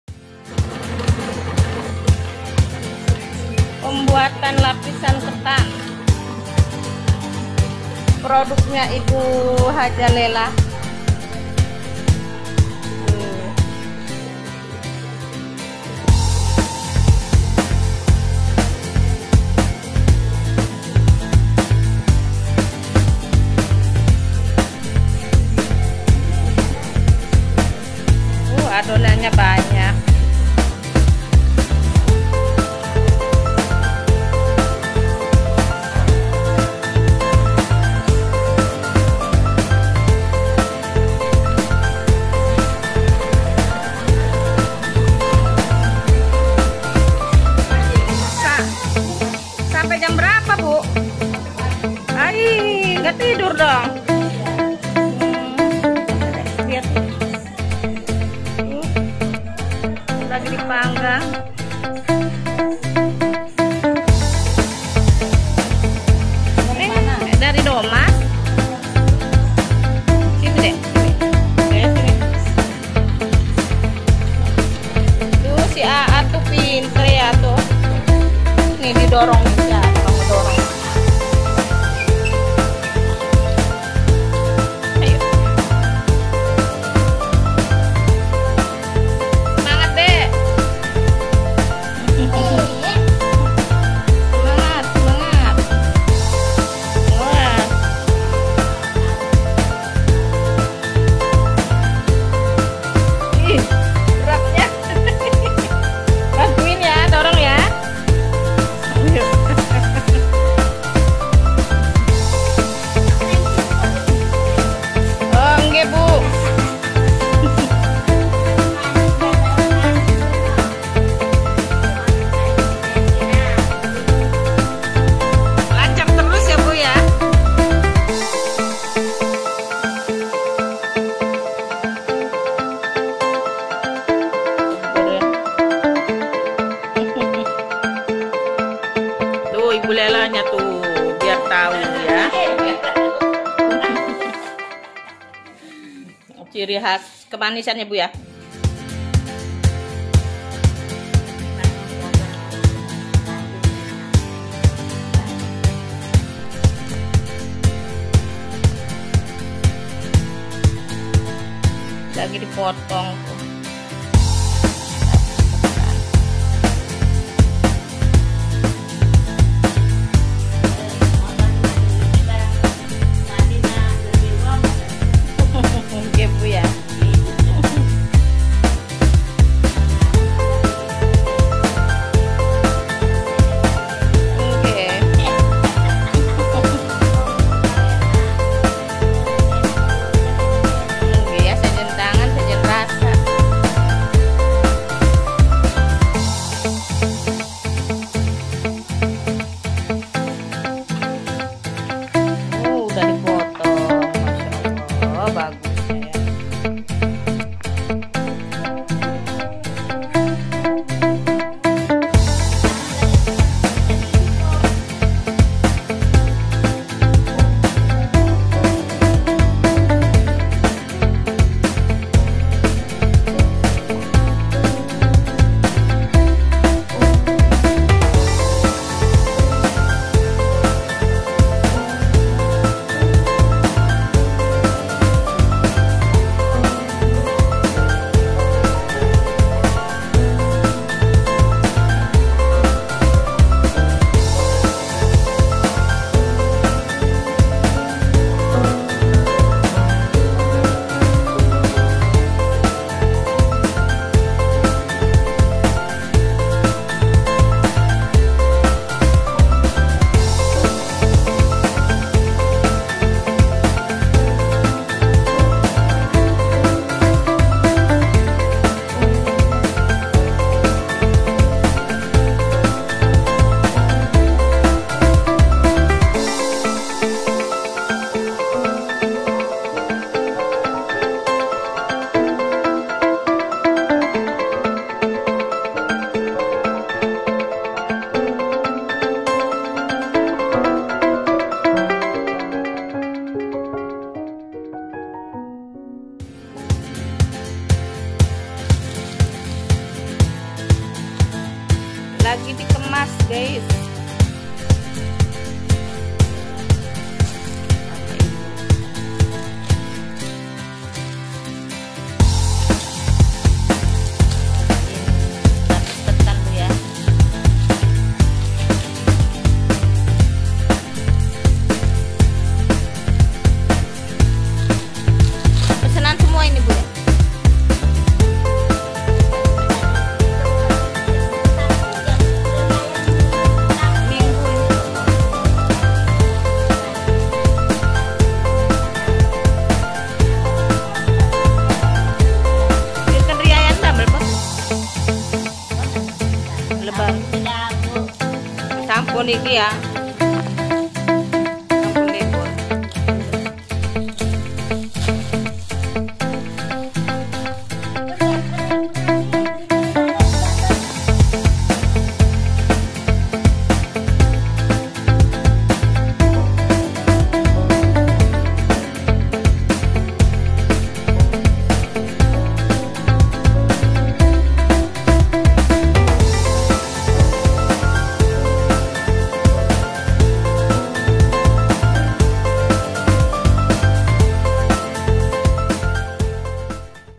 mengunjungi Umkm Lapis Ketan di wilayah kecamatan pontang kabupaten Serang,
Umkm Home industry Lapis ketan,
Umkm olahan kue kering dan
kue Basah,